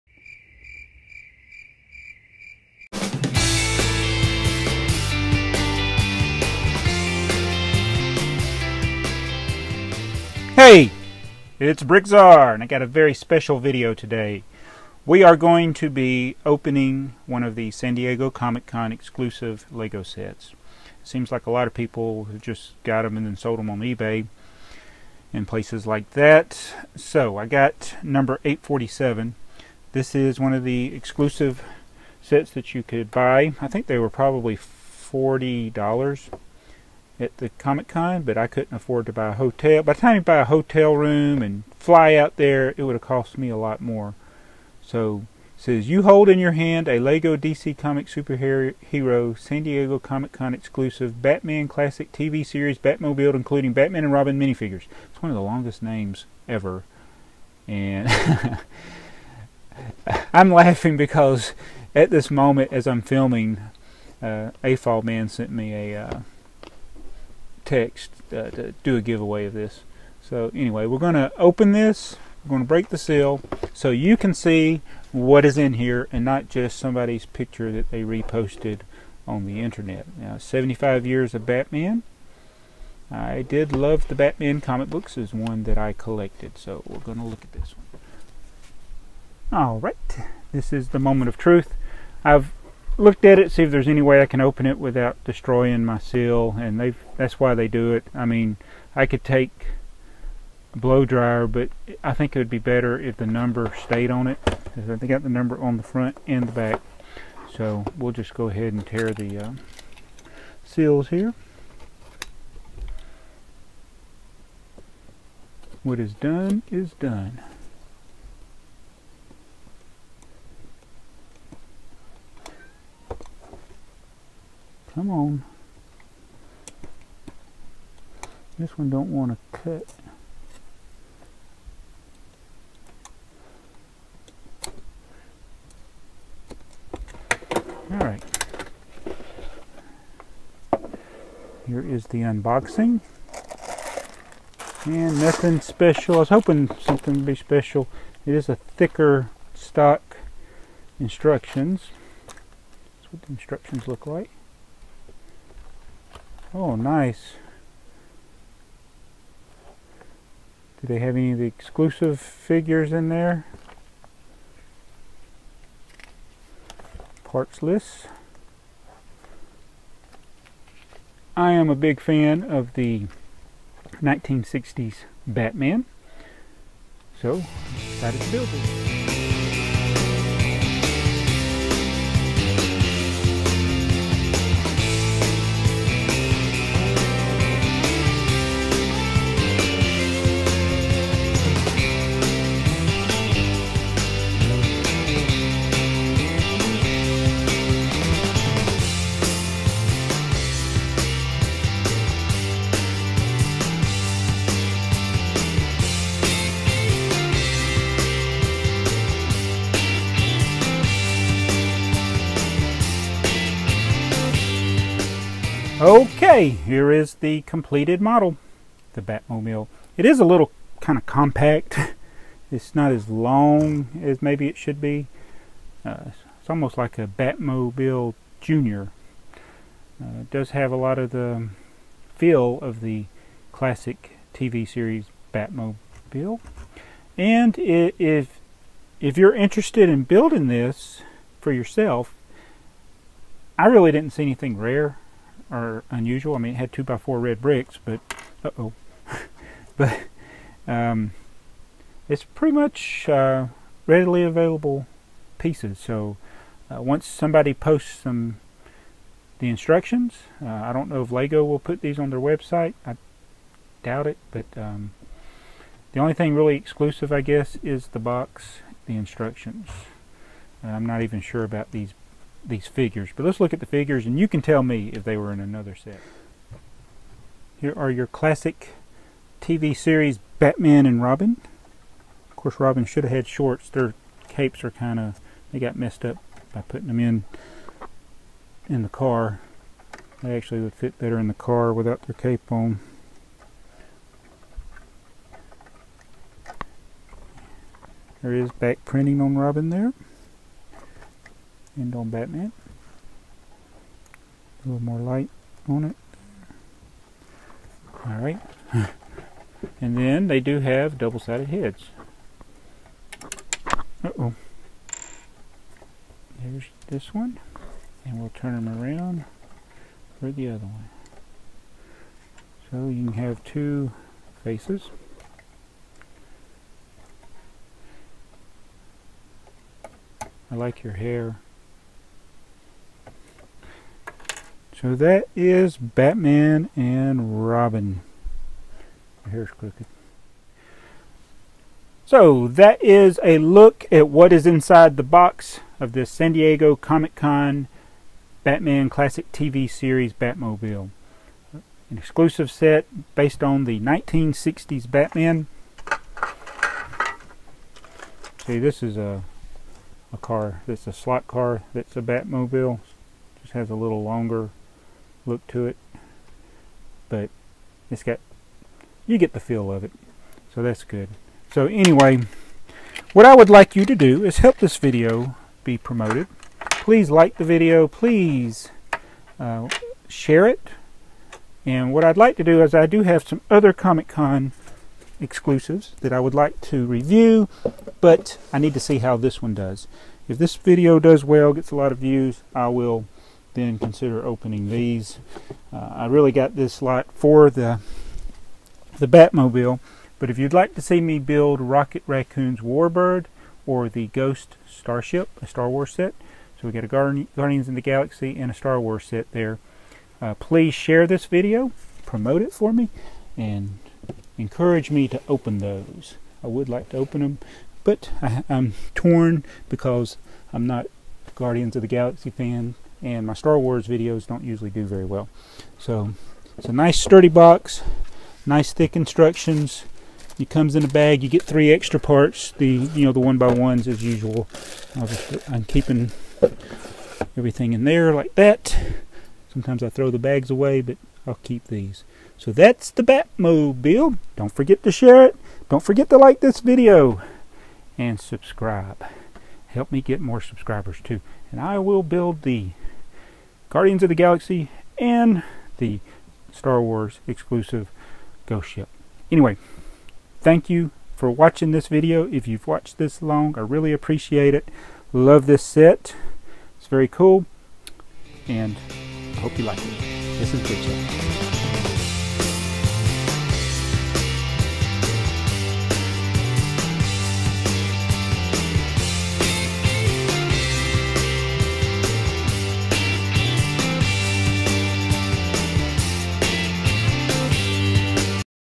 SDCC LEGO Batman Classic TV Series Batmobile - In Hand review!
SDCC LEGO Batman Classic TV Series Batmobile - In Hand review! 152 pieces - seems to be fairly common parts for the Batmobile. DC Comics Batman. See the actual set - not just a copy and paste from the internet!